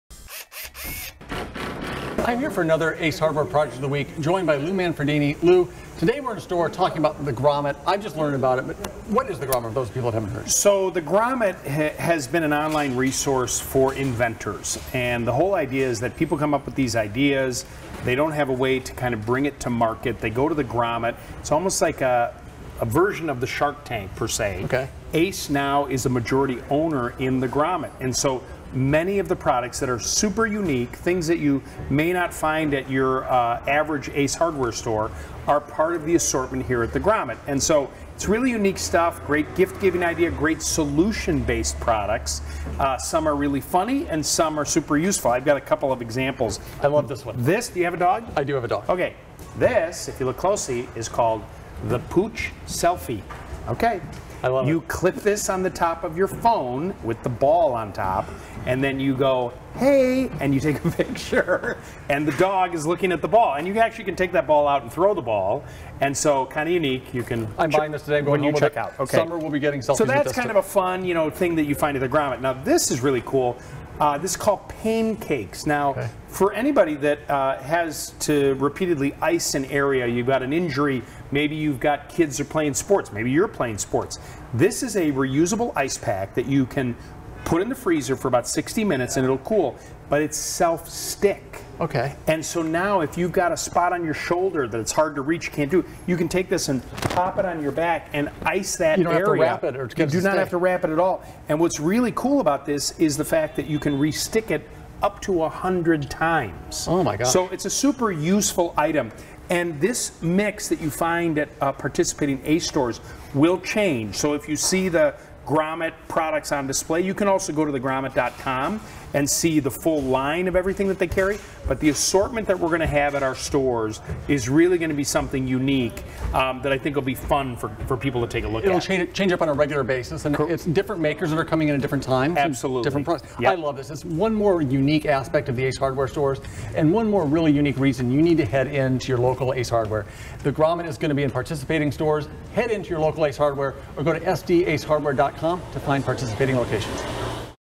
What Is The Grommet?
The Grommet is a resource for inventors to get their unique products to market. Each Ace Hardware carries a different assortment of products and they are constantly changing, so head in to your local Ace Hardware to find something unique.